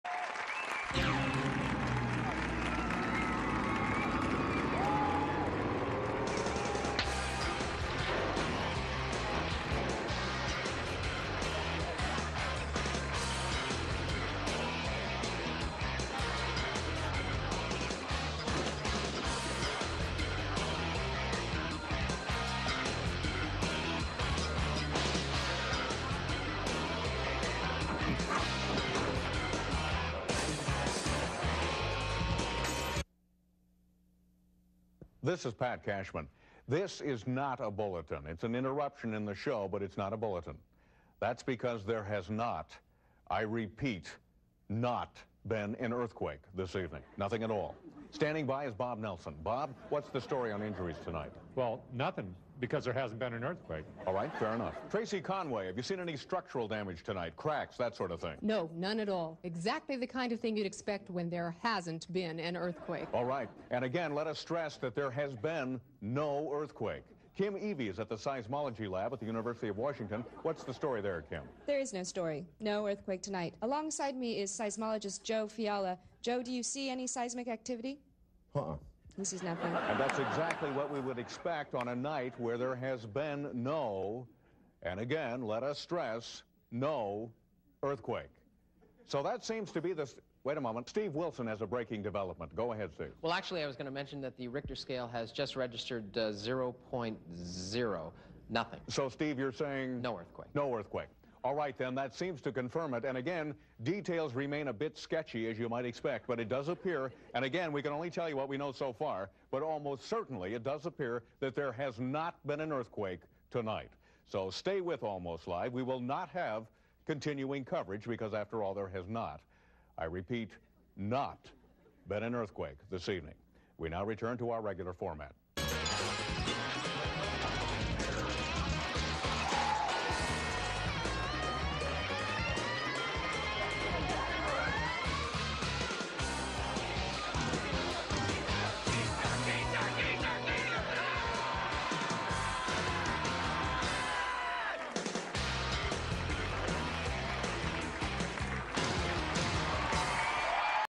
Bulletin - No Earthquake
This weathered wall opening was interrupted by an important message about the latest non-event.  Details are sketchy, but Pat has them.

First aired February 6, 1995